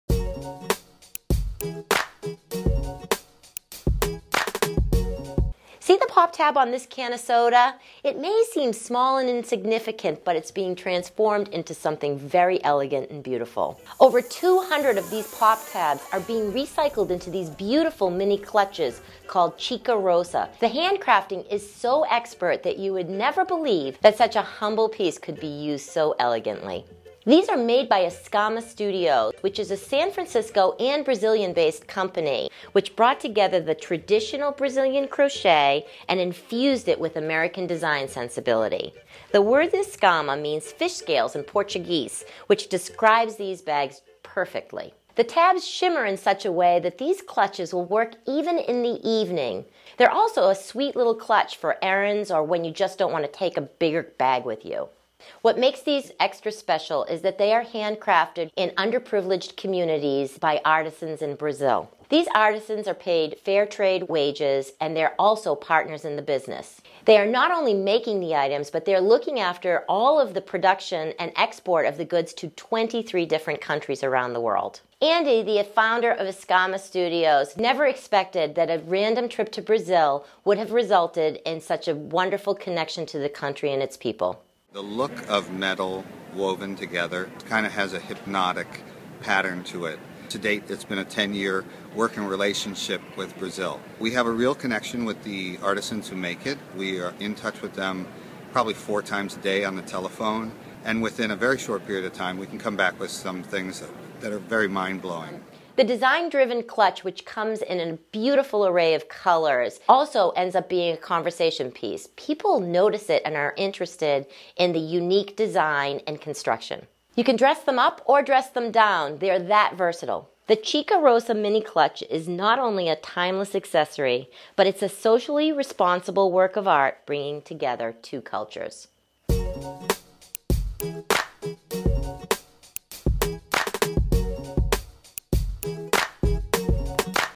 Escama Studio - Recycled Pull Tab Clutch
The Daily Grommet team discovers unique handbags, silver clutches from Escama. This recycled pull tab clutch is handcrafted, made in Brazil, in impoverished communities.